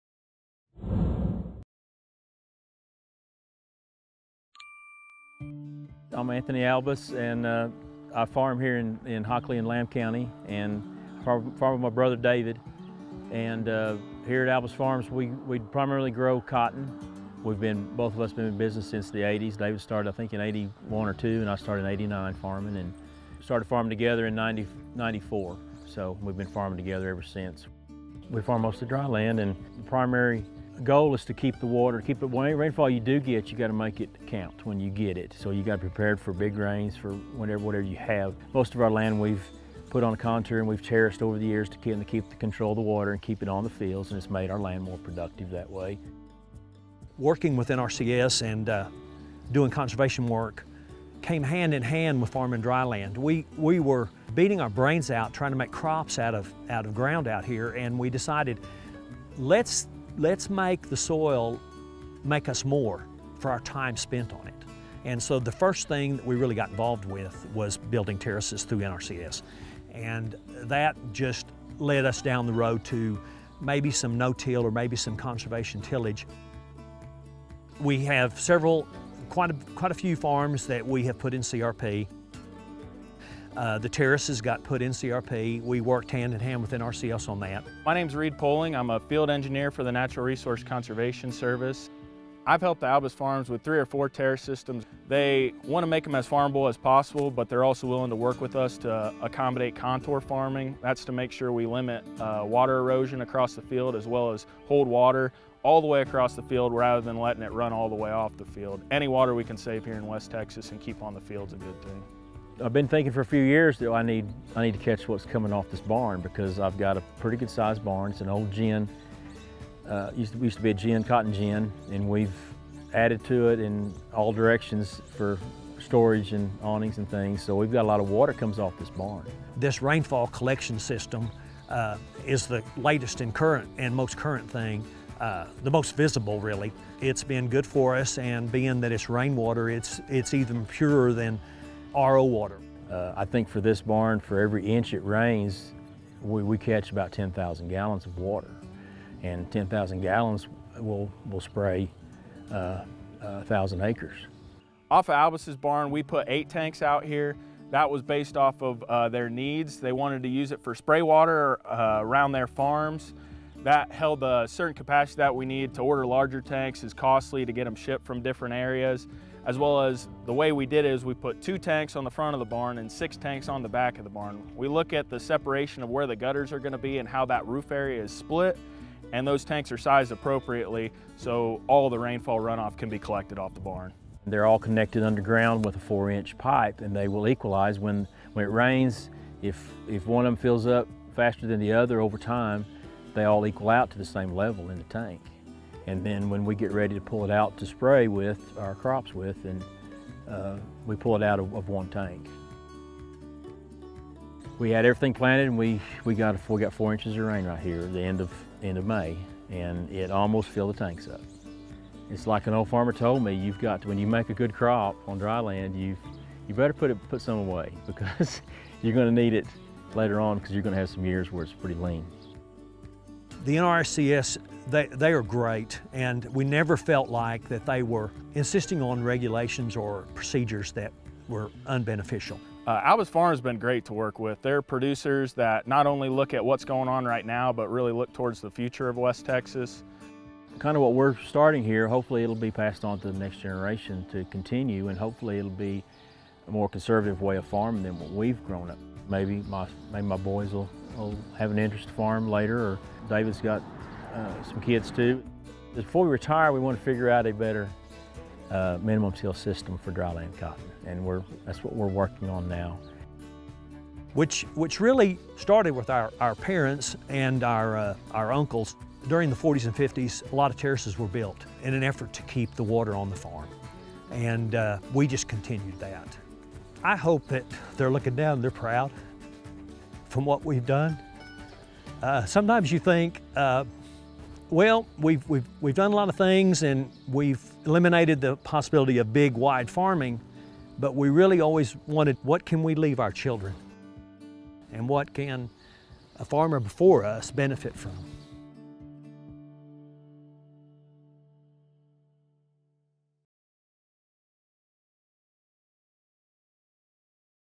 RAINWATER HARVESTING SYSTEM HELPS DRYLAND FARMERS ON THE SOUTH PLAINS
Albus Farms in Hockley County installs Rainwater Harvesting System for the purpose of spraying dryland cropland.  Anthony and David Albus work with NRCS through the EQIP program for system design and technical and financial assistance.